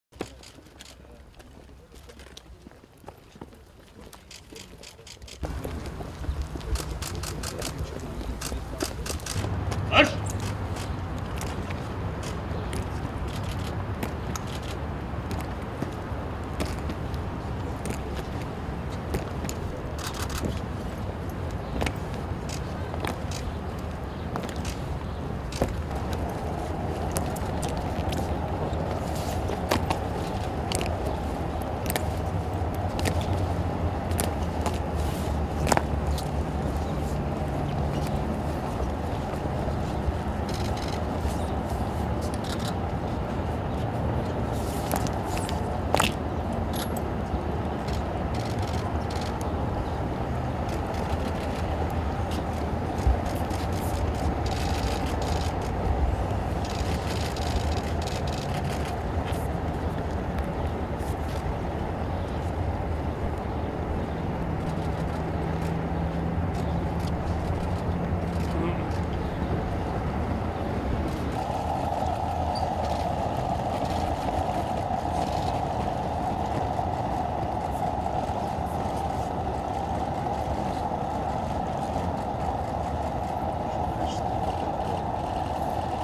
С.Лавров на церемонии возложения венка к Вечному огню мемориала павшим в ВОВ, 6 мая 2021 года
Министр иностранных дел Российской Федерации С.В.Лавров на церемонии возложения венка к Вечному огню мемориала павшим в Великой Отечественной Войне, Ереван, 6 мая 2021 года